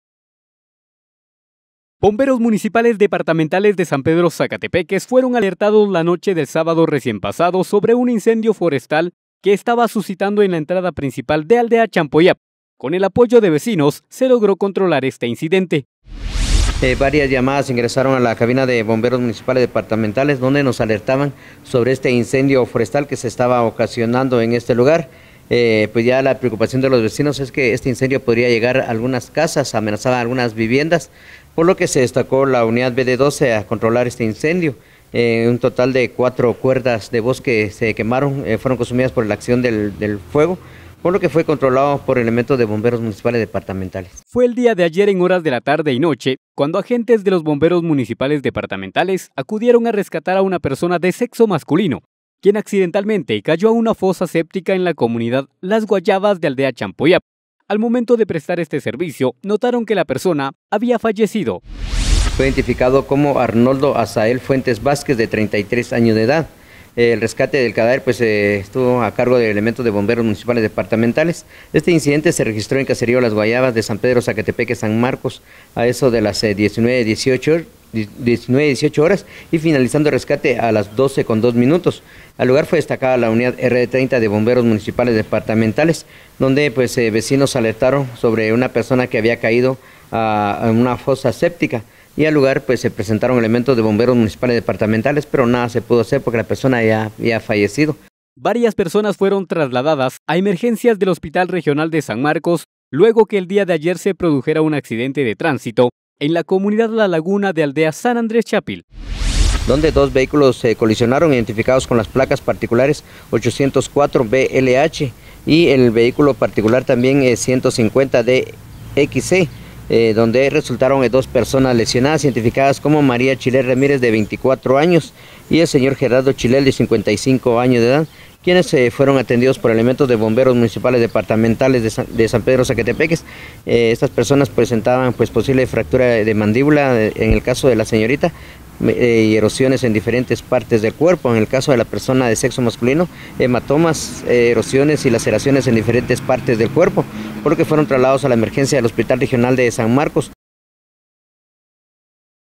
Emergencias cubiertas por cuerpos de socorro.
Bomberos Municipales Departamentales de San Pedro Sacatepéquez  fueron alertados la noche del sábado recién pasado sobre un incendio forestal que se estaba suscitando en la entrada principal de aldea Champollap, con el apoyo de vecinos se logro controlar este incidente.